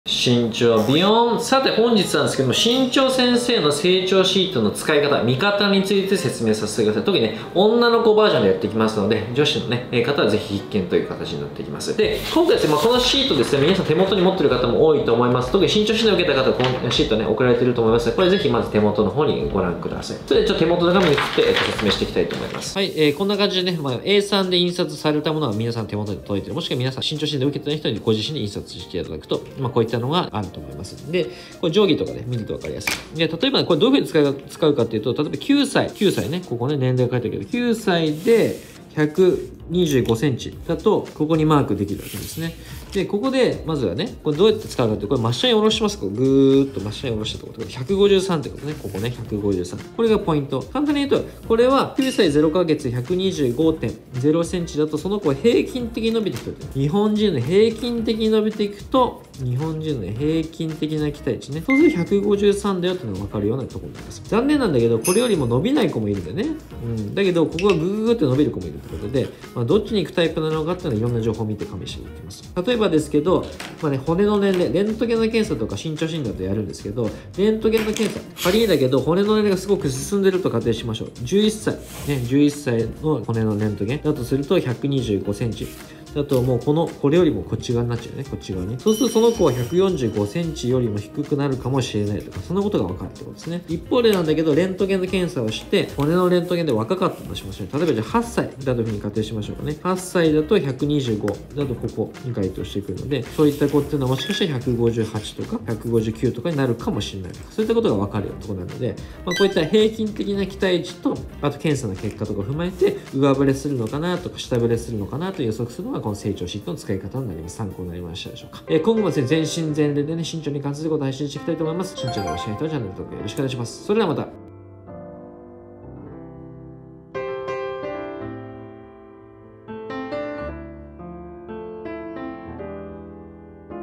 【女の子】身長先生の成長シートの見方を知ろう！【身長先生】
■目次
0:00 オープニング
0:03 今回のテーマ
0:23 成長シートの見方